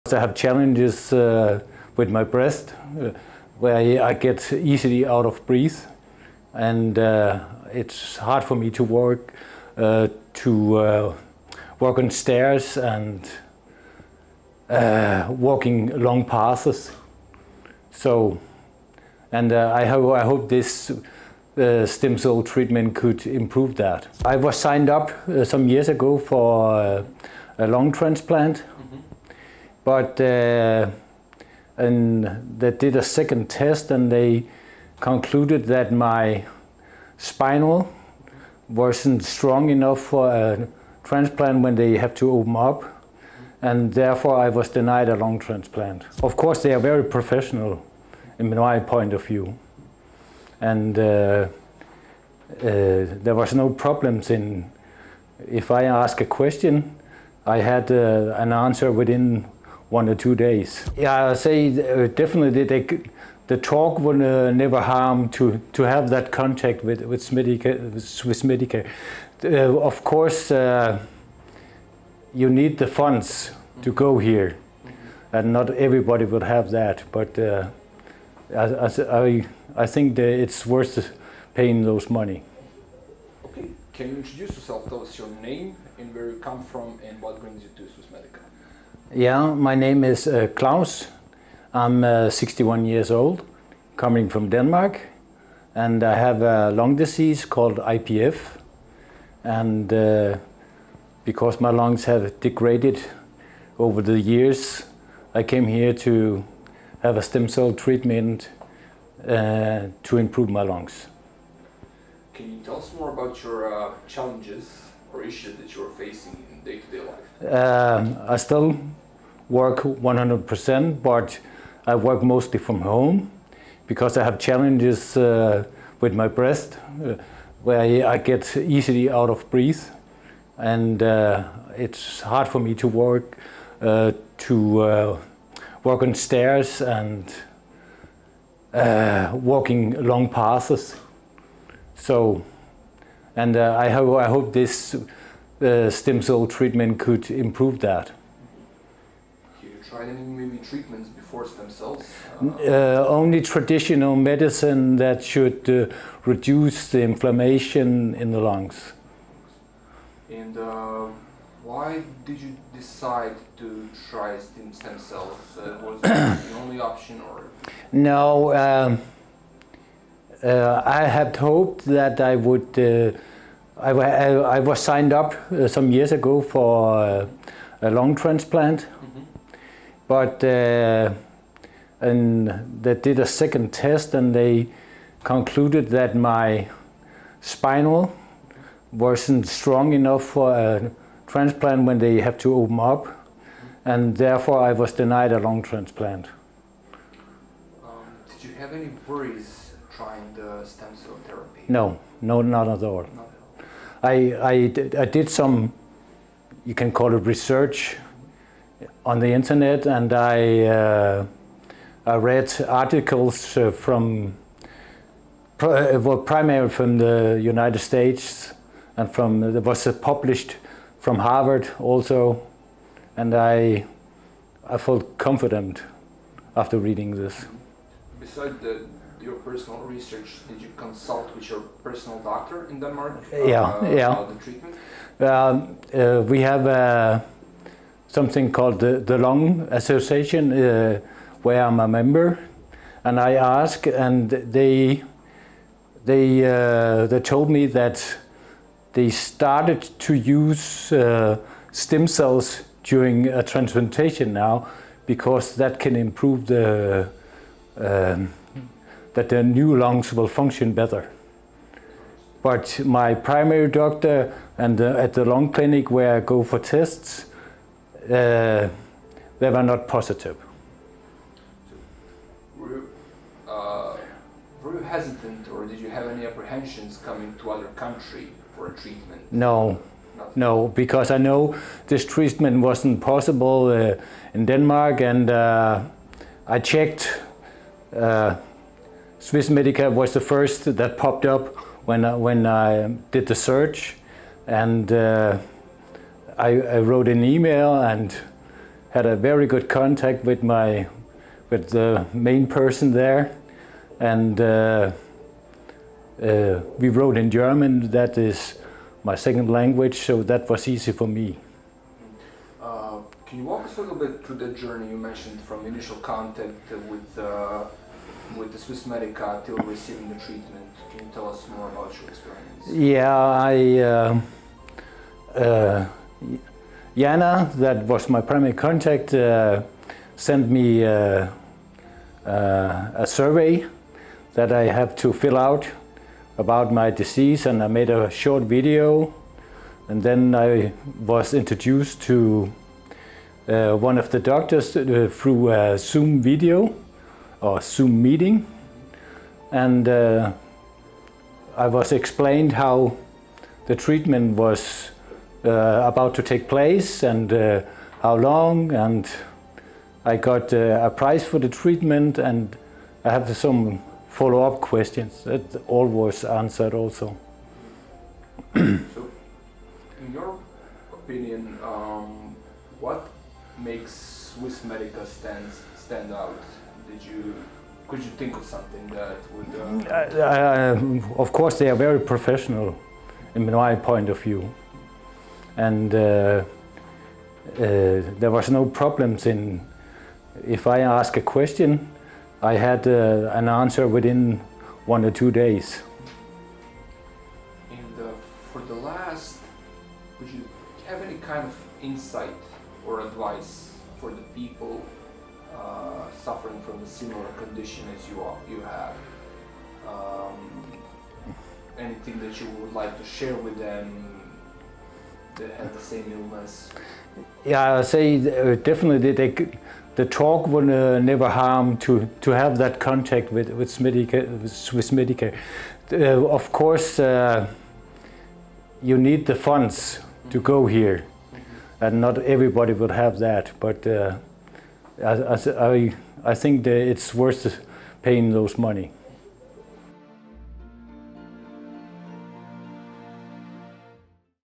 Stem Cells for Pulmonary Fibrosis: Story of 61-year-old Patient from Denmark
Our patient, a 61-year-old from Denmark, lives with a chronic lung condition called idiopathic pulmonary fibrosis (IPF). Over the years, his lungs have slowly deteriorated, making it harder for him to breathe. Even walking long distances or climbing stairs leaves him short of breath. Although the patient still works full-time, he mostly works from home due to these physical challenges.

Years ago, he was evaluated for a lung transplant. Unfortunately, further testing revealed that his spine wasn’t strong enough to safely undergo surgery. With that, the option of a transplant was off the table.
Looking for alternatives, the patient began researching regenerative therapies. He came across promising studies, including publications from Harvard, that gave him confidence in stem cell treatment. After reading about Swiss Medica clinic, he decided to reach out. What stood out immediately was the fast, professional response — in German, his second language — and the clear, informative consultation process.

While his local lung specialists in Denmark weren’t supportive — stem cell therapy for IPF isn't offered there — the patient learned from the Danish Lung Association that stem cells are now being used to support lung transplants in some cases.

The patient decided to go forward with the treatment, hopeful it would reduce his lung inflammation and help him breathe more easily. He emphasizes that while treatment abroad may not be an option for everyone, reaching out and learning about alternatives is always worth it.

💬  “If you have the chance,” he says, “it’s worth it.”

𝘗𝘭𝘦𝘢𝘴𝘦 𝘯𝘰𝘵𝘦: 𝘛𝘩𝘦 𝘤𝘰𝘯𝘵𝘦𝘯𝘵 𝘱𝘳𝘰𝘷𝘪𝘥𝘦𝘥 𝘪𝘯 𝘵𝘩𝘪𝘴 𝘷𝘪𝘥𝘦𝘰 𝘪𝘴 𝘯𝘰𝘵 𝘪𝘯𝘵𝘦𝘯𝘥𝘦𝘥 𝘢𝘴 𝘮𝘦𝘥𝘪𝘤𝘢𝘭 𝘢𝘥𝘷𝘪𝘤𝘦. 𝘐𝘯𝘥𝘪𝘷𝘪𝘥𝘶𝘢𝘭 𝘵𝘳𝘦𝘢𝘵𝘮𝘦𝘯𝘵 𝘳𝘦𝘴𝘶𝘭𝘵𝘴 𝘮𝘢𝘺 𝘷𝘢𝘳𝘺. 𝘈𝘭𝘸𝘢𝘺𝘴 𝘤𝘰𝘯𝘴𝘶𝘭𝘵 𝘸𝘪𝘵𝘩 𝘢 𝘲𝘶𝘢𝘭𝘪𝘧𝘪𝘦𝘥 𝘩𝘦𝘢𝘭𝘵𝘩𝘤𝘢𝘳𝘦 𝘱𝘳𝘰𝘧𝘦𝘴𝘴𝘪𝘰𝘯𝘢𝘭 𝘳𝘦𝘨𝘢𝘳𝘥𝘪𝘯𝘨 𝘢𝘯𝘺 𝘮𝘦𝘥𝘪𝘤𝘢𝘭 𝘤𝘰𝘯𝘥𝘪𝘵𝘪𝘰𝘯.